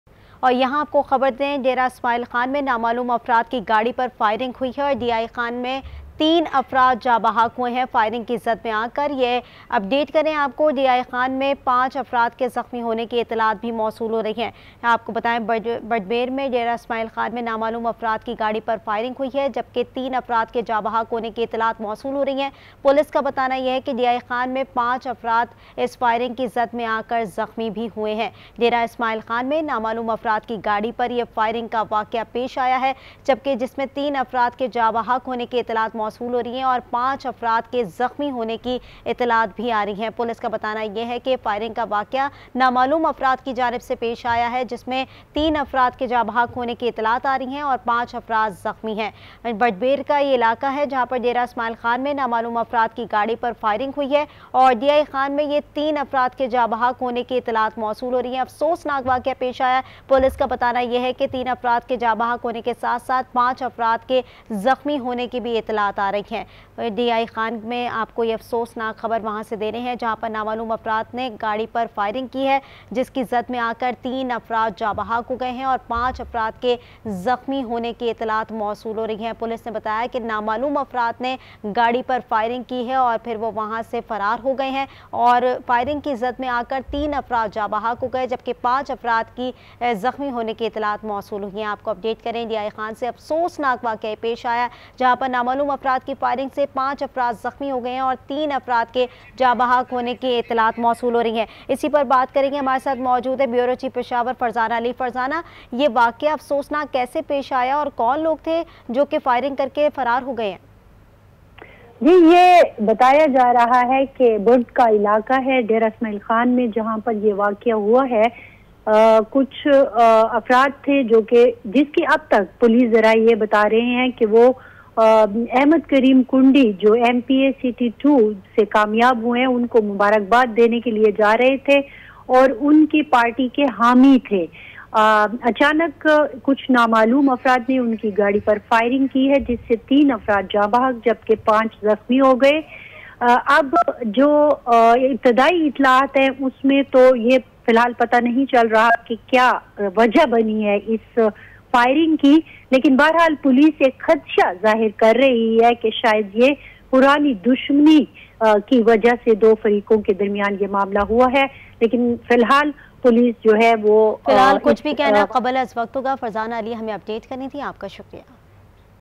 Major firing incident in Dera Ismail Khan - Aaj News
Major firing incident in Dera Ismail Khan - 12 February, 2024  - Aaj News

Spot Light With Munizae Jahangir:
shorturl.at/gwFU6